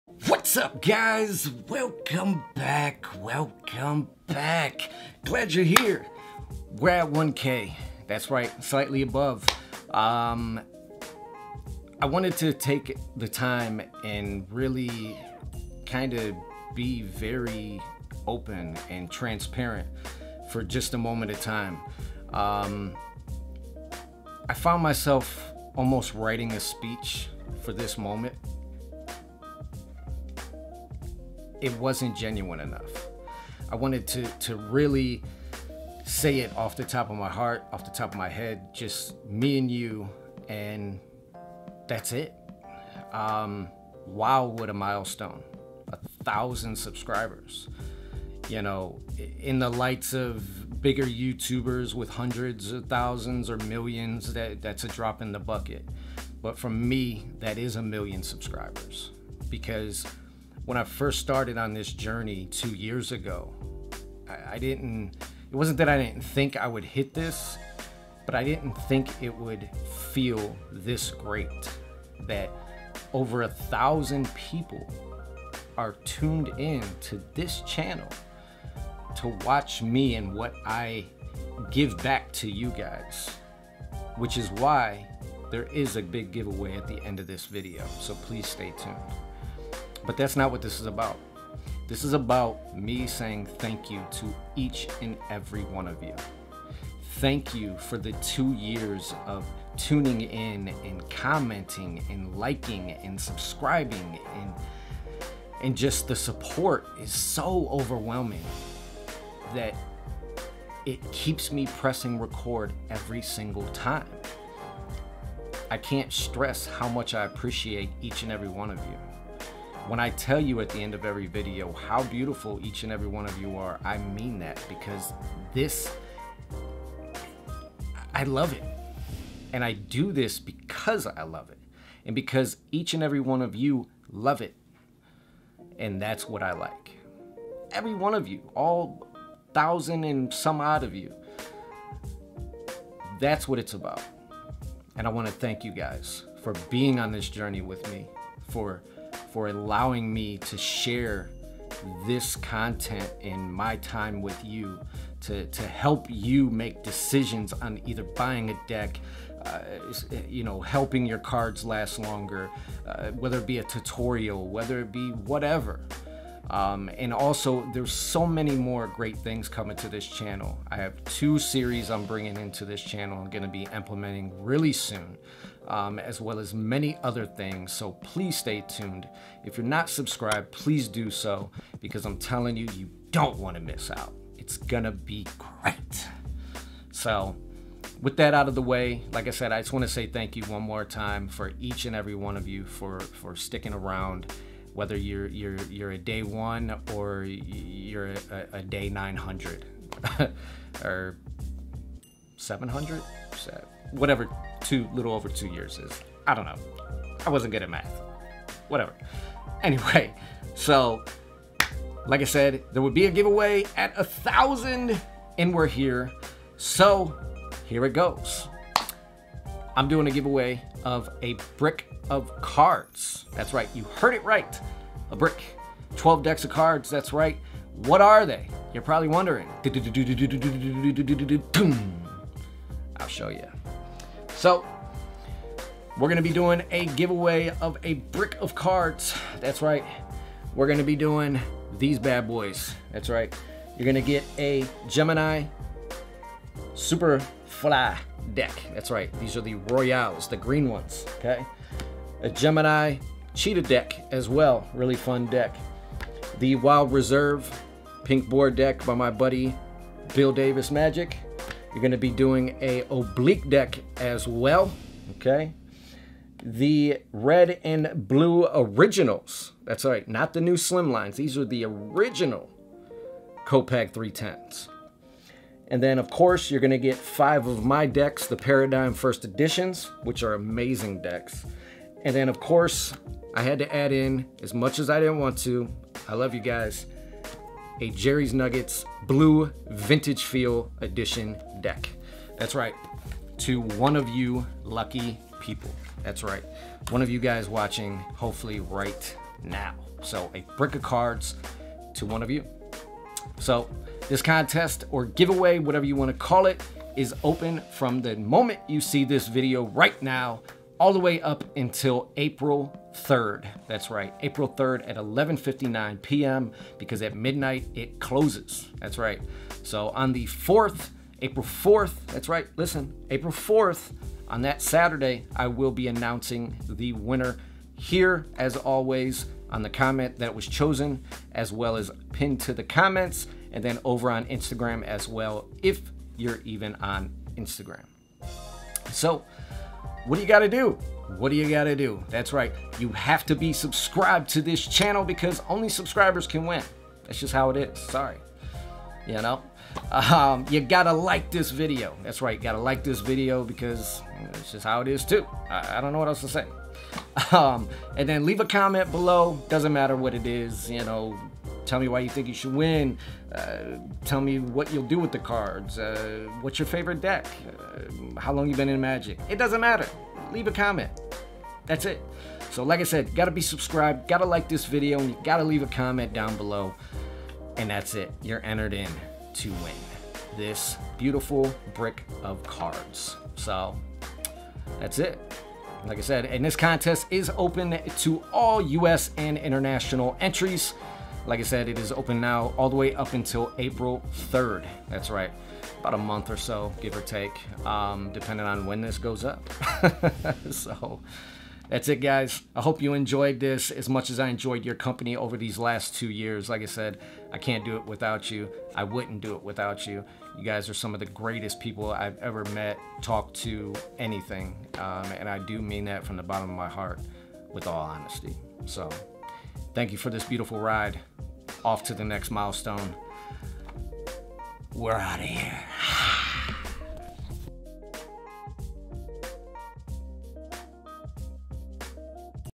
1000 SUBSCRIBERS Thank You & 12 DECK GIVEAWAY!!! (CLOSED)
WOW! It almost doesnt seem real at all. I cant thank you all enough for such a huge milestone. Thank you for being part of this awesome journey over the past 2 years. Whether youve been here one day or two years, just know that I love you all more than I can ever express! So ... as a token of my appreviation and to say thank you Im doing a great giveaway of 12 decks of cards.

       Thats right! You heard it right. 12 decks. The biggest giveaway I ever did and just as I promised! So what 12 decks are up for grabs?

* Gemini Superfly Royale'
* Gemini Cheetah
* Wild Reserve: Pink Boar By: Bill Davis
* Oblique By: Card Cutz
* Copag 310 (Red) By: Cartamundi
* Copag 310 (Blue) By: Cartamundi
* (5)Paradigm (First Edition By: Disfunkshinal Reviews
*Jerrys Nugget (Blue) (Vintage Feel) By: EPCC

*Giveaway is OPEN from NOW and will run until April 3rd, 2020 at 11:59pm (EST). Giveaway Will Officially CLOSE at 12:00am (EST) on April 4th, 2020

* Open to all entries with a valid account in the U.S. and International

"Thats it"... and your entered!
What I filmed this video with:

Main Camera:

Lens #1 :

Lens #2 :

Microphone :
THANK YOU ALL and CHEERS to the next milestone! I love you all!